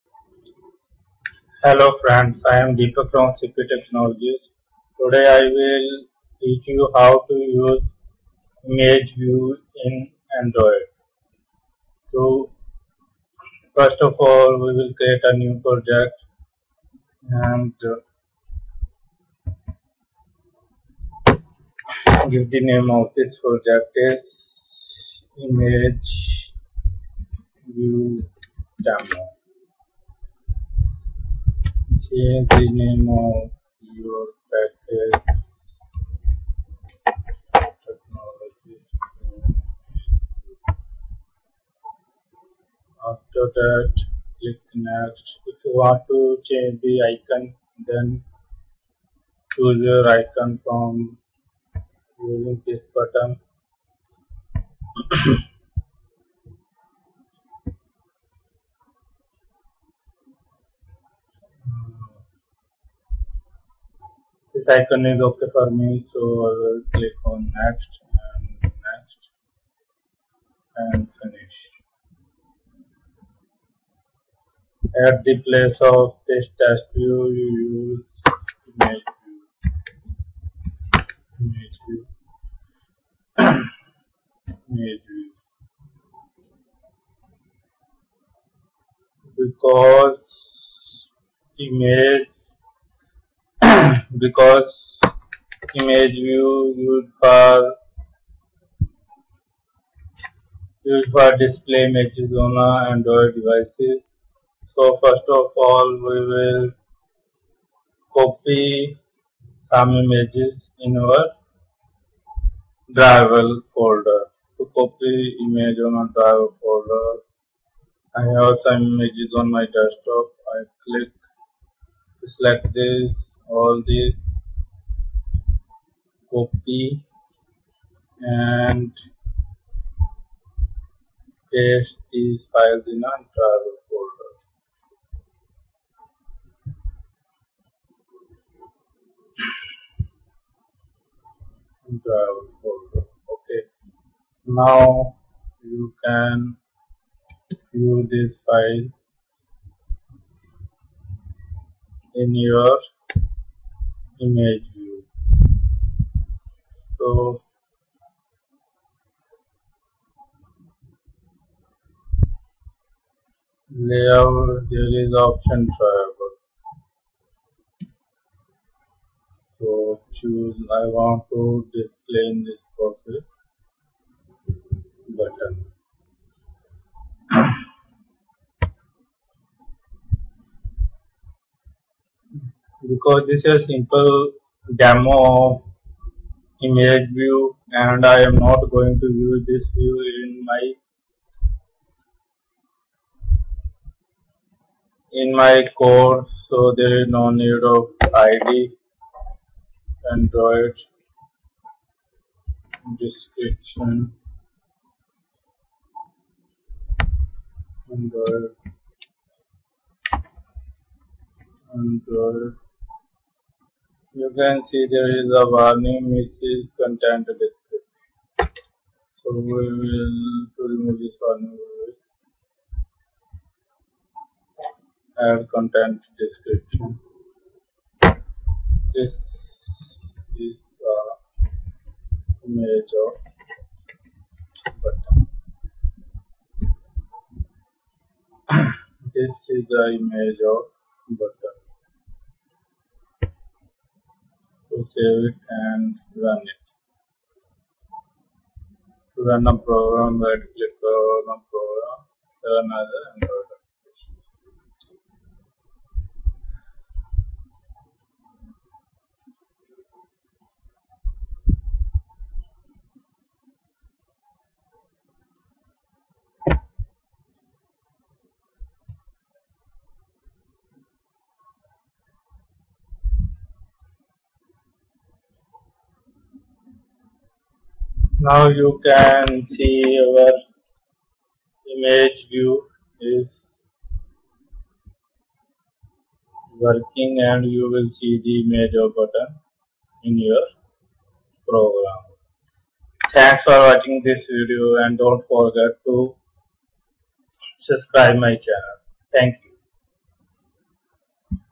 ImageView in Android
In this video you will learn how to use image view in android to display images in android. Please Download source file of this project